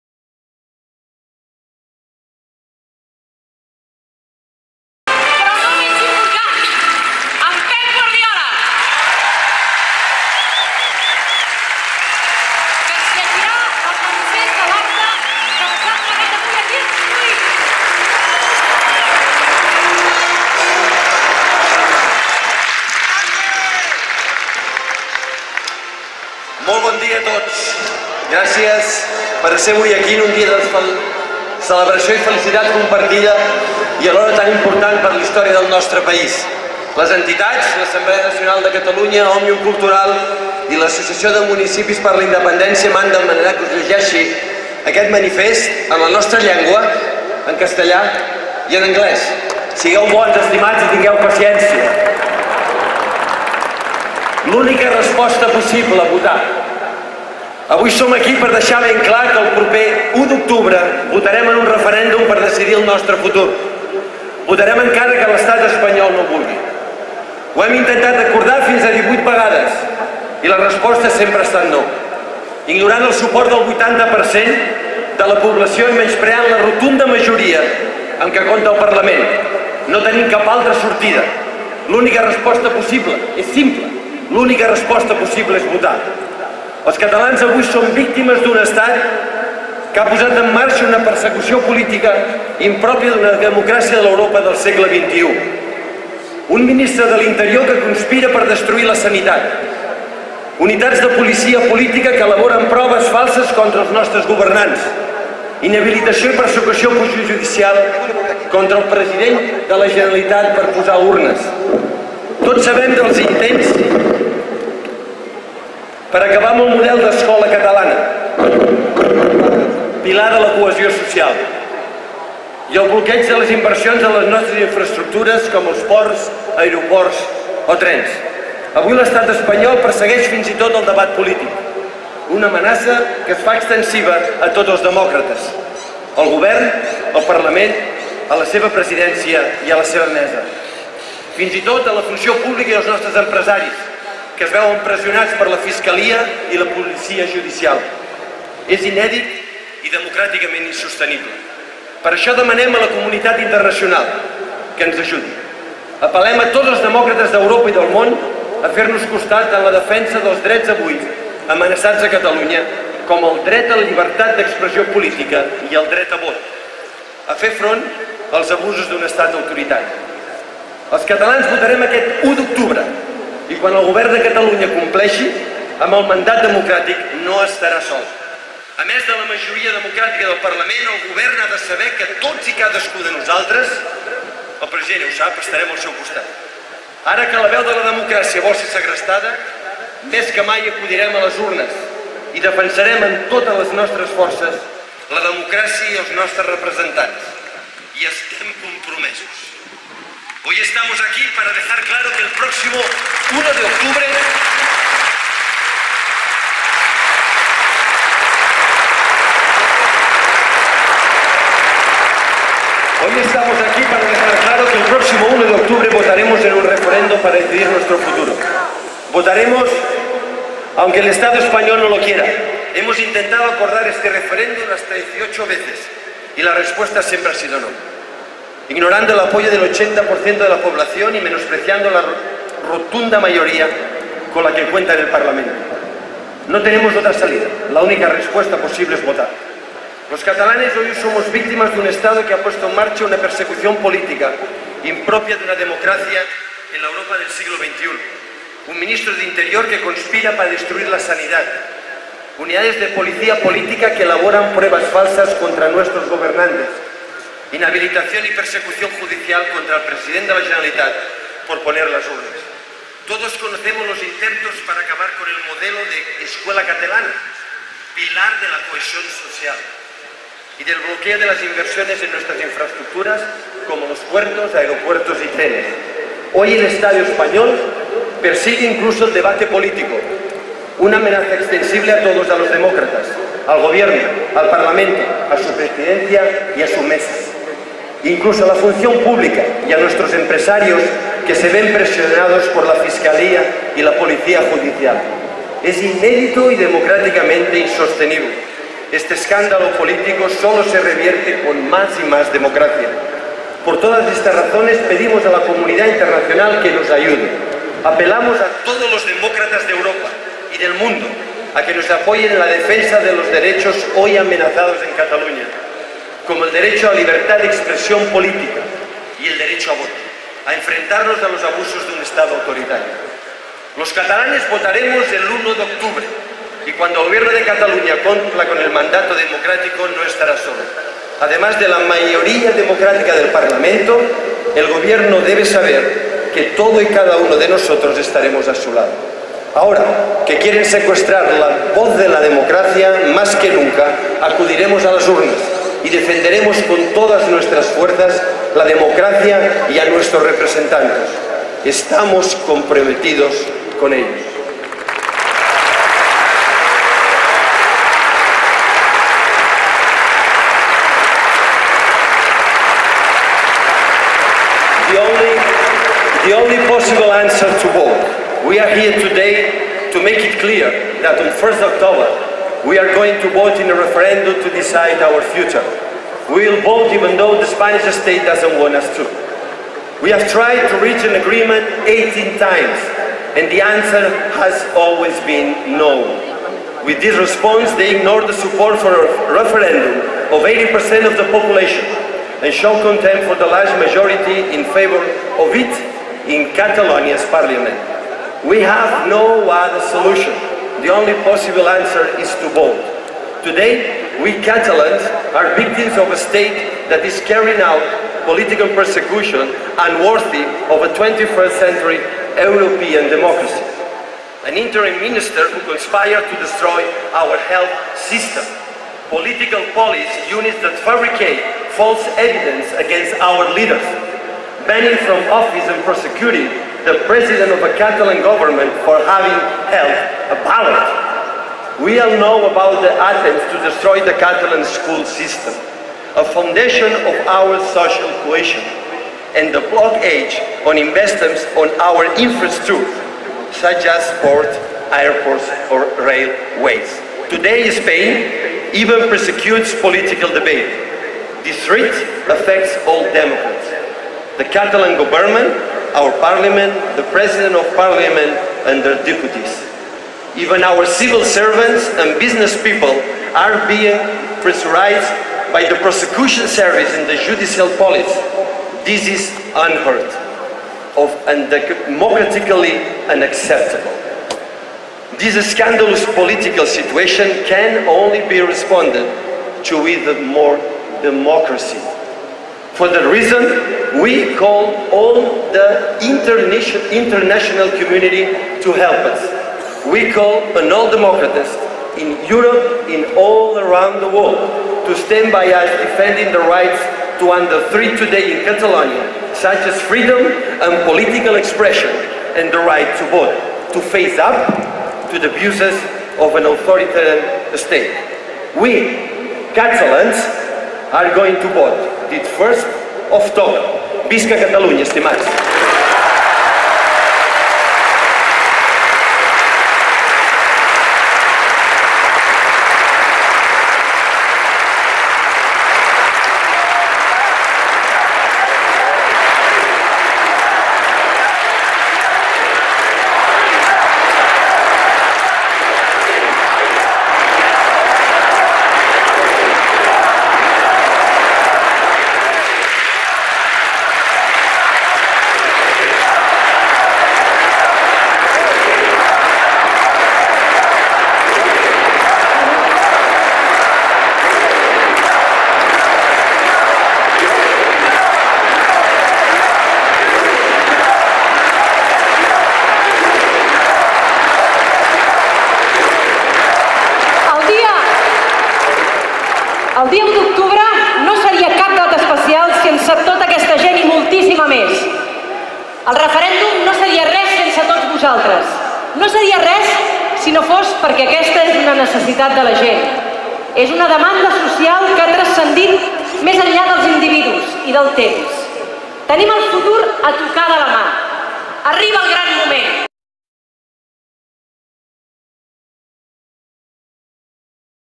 Josep Guardiola amb la Generalitat i per Referèndum és Democràcia!.
«Votarem encara que l'estat espanyol no ho vulgui»
L'entrenador llegeix en català, castellà i anglès el manifest de la concentració sobiranista i reclama ajuda "a la comunitat internacional"

Aquí el pots llegir íntegrament:

'L'única resposta possible: votar'

Avui som aquí per deixar ben clar que el proper 1 d'octubre votarem en un referèndum per decidir el nostre futur. Votarem encara que l'Estat espanyol no ho vulgui.

Ho hem intentat acordar aquest referèndum fins a 18 vegades i la resposta sempre ha estat No, ignorant el suport del 80% de la població i menyspreant la rotunda majoria amb la qual explica al Parlament. No tenim una altra sortida. L'única resposta és votar.

Els catalans avui som víctimes d'un Estat que ha posat en marxa una persecució política impròpia d'una democràcia en l'Europa del segle XXI.

Un ministre de l'Interior que conspira per destruir la sanitat, unitats de policia política que elaboren proves falses contra els nostres governants, inhabilitació i persecució judicial contra el president de la Generalitat per posar les urnes.

Tots coneixem els intents per acabar amb el model d'escola catalana, pilar de la cohesió social. I del bloqueig de les inversions en les nostres infraestructures, com els ports, l'aeroport o els trens...

Avui l'Estat espanyol persegueix fins i tot el debat polític. Una amenaça extensible a tots els demòcrates: al Govern, al Parlament, a la seva presidència i a la seva mesa. Fins i tot, a la funció pública i als nostres empresaris que es veuen pressionats per la fiscalia i la policia judicial. És inèdit i democràticament insostenible.

Aquest escàndol polític solament es reverteix amb més democràcia. Per això  demanem a la comunitat internacional que ens ajudi. Apel·lem a tots els demòcrates d'Europa i del món a fer-nos costat en la defensa dels drets avui amenaçats a Catalunya com el dret a la llibertat d'expressió política i el dret a vot. A fer front als abusos d'un estat autoritari.

Els catalans votarem aquest 1 d'octubre, i quan el Govern de Catalunya compleixi amb el mandat democràtic no estarà sol.

A més, de la majoria democràtica del Parlament, el Govern ha de saber que tots i cadascun de nosaltres estarem al seu costat.

Ara que la veu de la democràcia vol ser segrestada, més que mai acudirem a les urnes i defensarem amb totes les nostres forces la democràcia i els nostres representants. Hi estem compromesos. No fallarem!

Les frases més destacades del manifest de Pep Guardiola

"Votarem encara que l'Estat espanyol no ho vulgui. Hem intentat acordar aquest referèndum fins a 18 vegades i la resposta sempre ha estat que no. Ignorant el 80% de la població i menyspreant la rotunda majoria amb la qual compta el Parlament. No tenim una altra sortida: l'única resposta és votar".

"Els catalans som víctimes d'un Estat que ha posat en marxa una persecució política impròpia d'una democràcia de l'Europa del segle XXI".

"Tots coneixem els intents per acabar amb el model de l'escola catalana i el bloqueig de les inversions en infraestructures".

"Avui l'Estat Espanyol fins i tot persegueix el debat polític. Per totes aquestes raons demanem a la comunitat internacional ens ajudi".

"Ara que volen segrestar la veu de la democràcia, més que mai, acudirem a les urnes i defendrem amb totes les nostres forces la democràcia i als nostres representats. Estem compromesos. No fallarem".